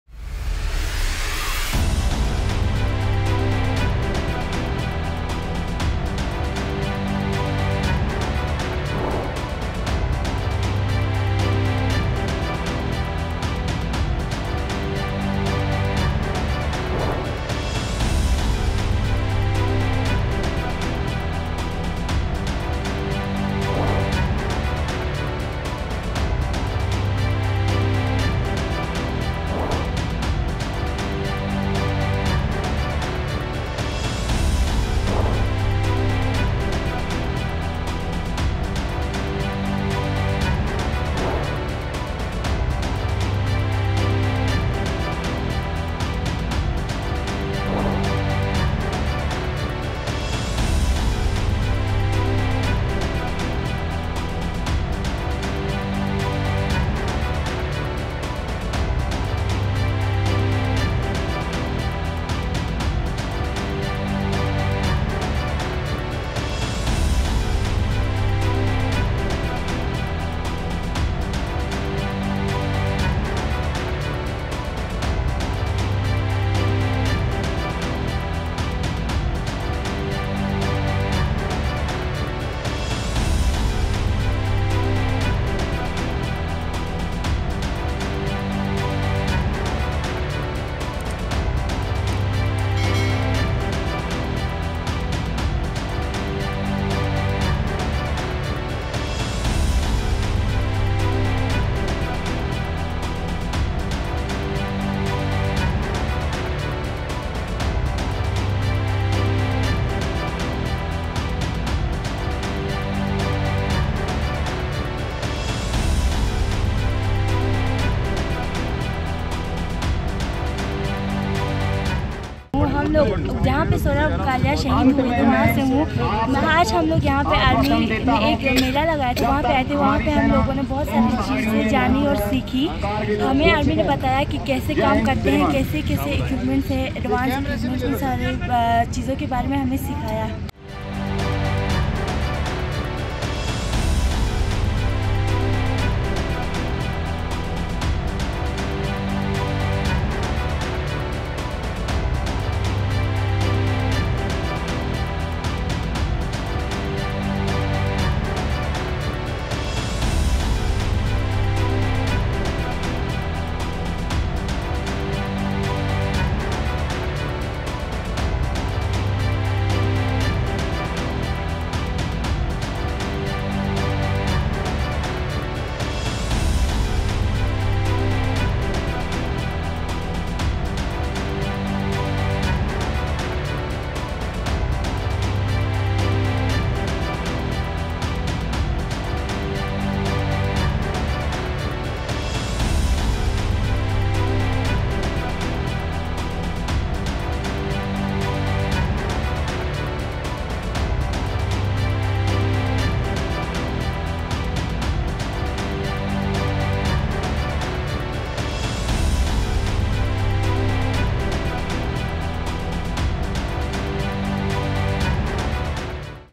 Enthralled students enjoy tank rides at the Kargil Vijay Diwas 'Know Your Army' show in Ladakh
In a grand prelude to the Rajat Jayanti of Kargil Vijay Diwas, Indian Army hosted a spectacular 'Know Your Army' Weapon display program at Kargil Helipad on July 13. The event exhibited an impressive showcase of latest weaponry and equipment from Indian Army and Indian Air Force. The event drew enthusiastic crowds, including children from various schools, colleges and NCC Cadets. Thrilling rides, such as military vehicle experiences and breath-taking hot air balloon rides, captivated the young attendees. The ‘Know Your Army’ Equipment Display successfully combined education, entertainment, and inspiration leaving a lasting impact on over 3,000 locals and children.